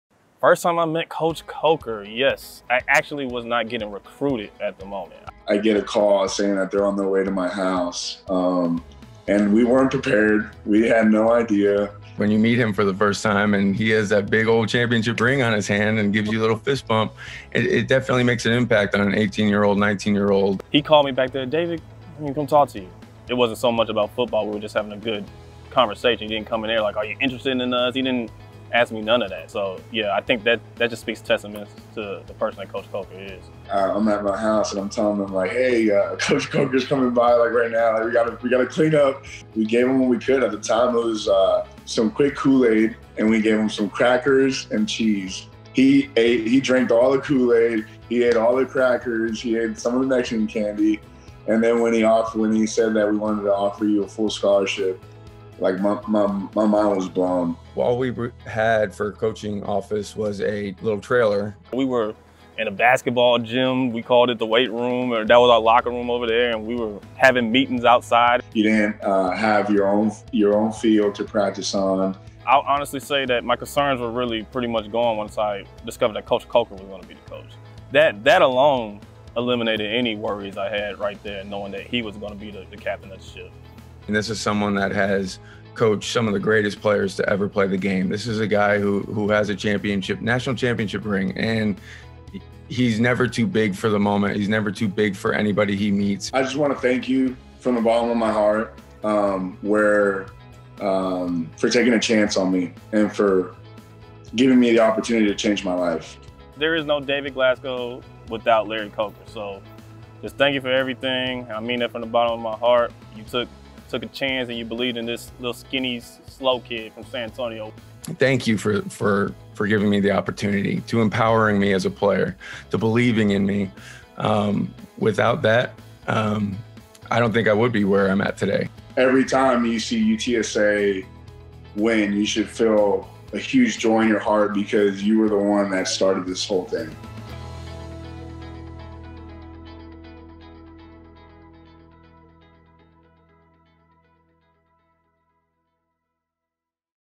Larry Coker First UTSA Football Head Coach Endowed Award
This month we are honoring UTSA's first head football coach, Larry Coker, with a special opportunity to show gratitude for his contributions to the university. By giving a gift, your support will honor Coach Coker and ensure his legacy continues to reach each new generation of Roadrunners, by helping deserving student-athletes with a strong work ethic pursue dreams of achieving a college education.